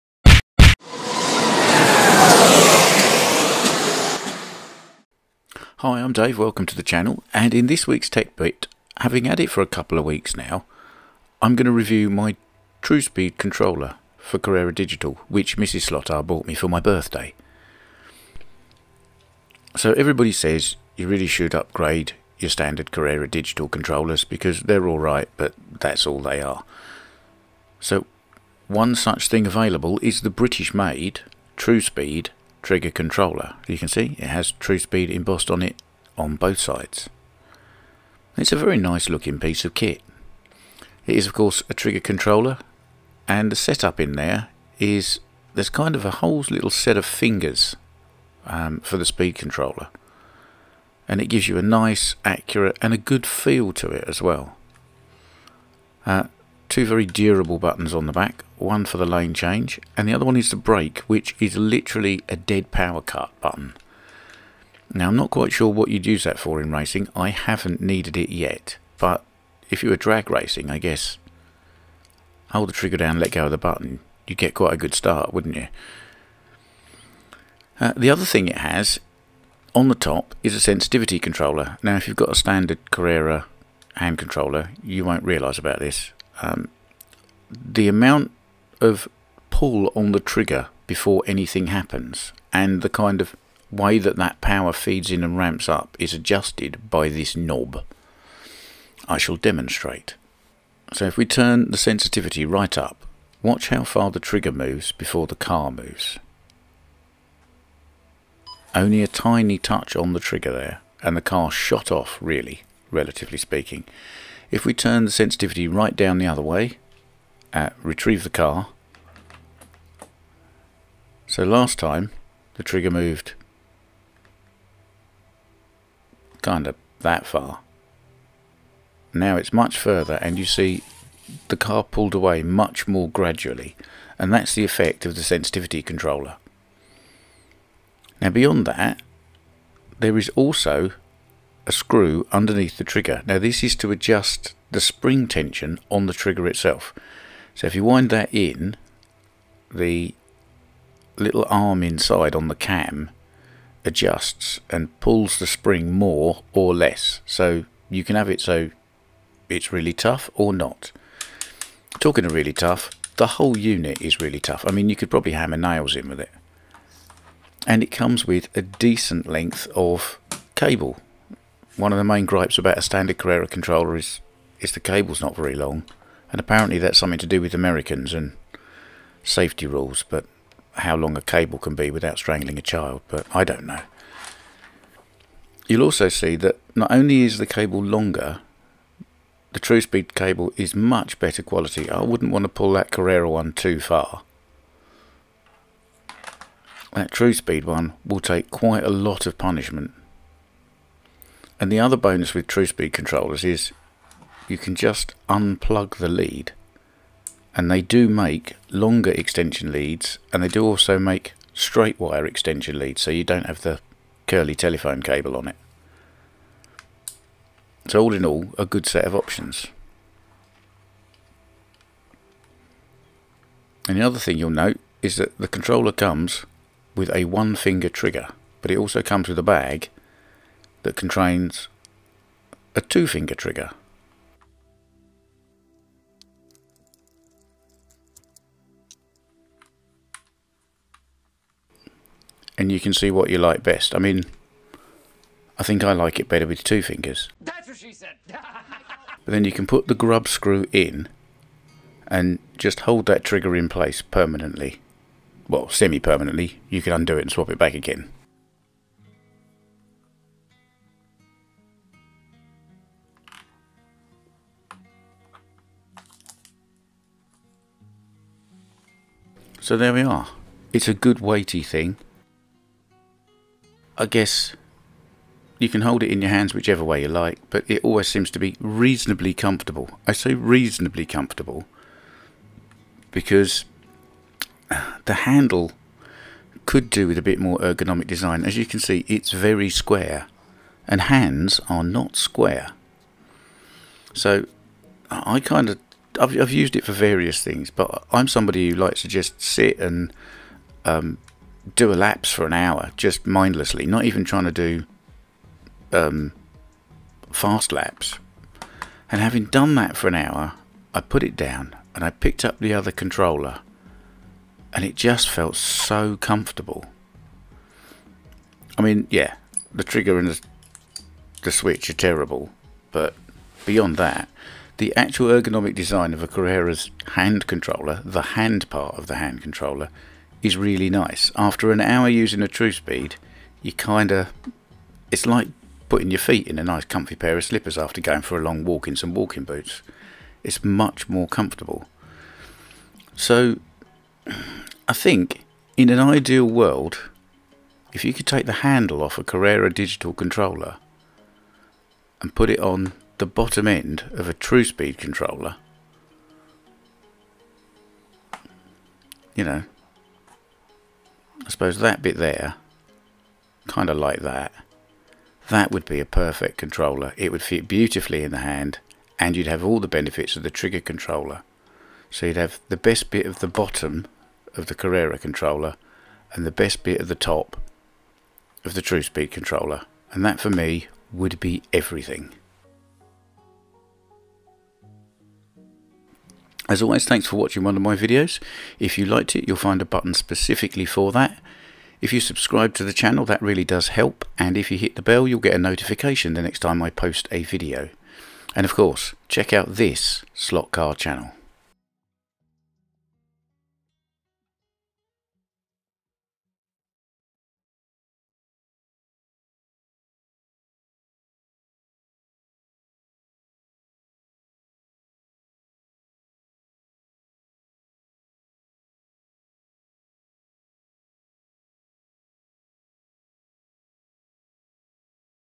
Truspeed Controller for Carrera Digital Slot Car Track
Mrs SlotR bought me a Truspeed hand controller for my birthday (thank you Mrs SlotR)
I've had it about a month now and I've used it a fair bit, so here's my quick review of it.